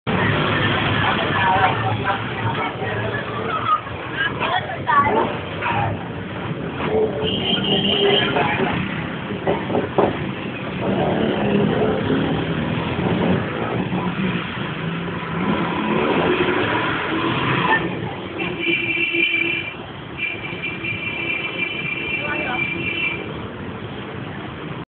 วางระเบิดแถวพันทิพ 1
แต่กระเป๋านี่มันคุ้นๆ ว่ามันเป็นของคนที่เคยมาขายของแถวนั้นนี่?